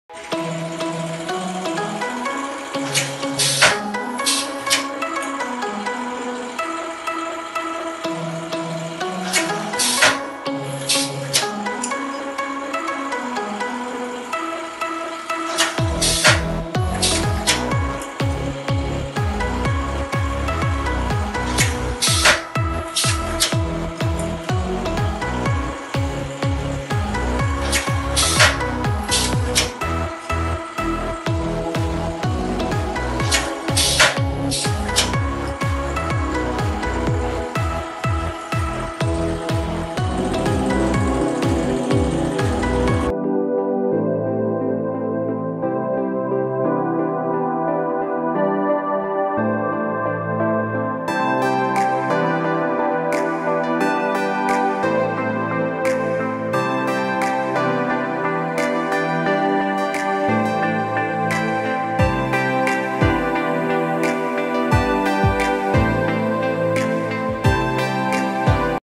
Saddle clip making machine Made In Sri Lanka
This is an one of our saddle clip making machine. We are providing customized solution's for your Business and Factory machine requirements.
Just Imagine We Make it Reality